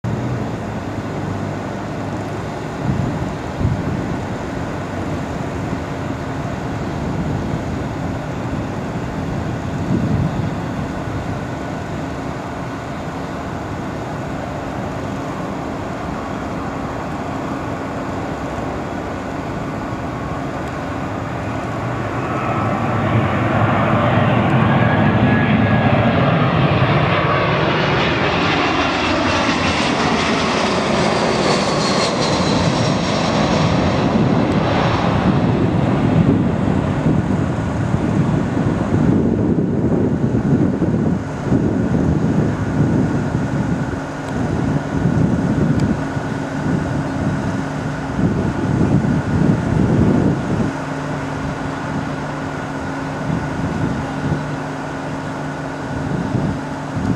Royal Jordanian RJ121 Airbus A319-132 JY-AYL final approach at Berlin Tegel airport yellow engine?
Airline: Royal Jordanian
IATA Code: RJ
ICAO Code: RJA
Callsign: JORDANIAN
Aircraft typ: Airbus A319-132
Registration: JY-AYL
Flight number: RJ121
Destination: Amman AMM - Berlin TXL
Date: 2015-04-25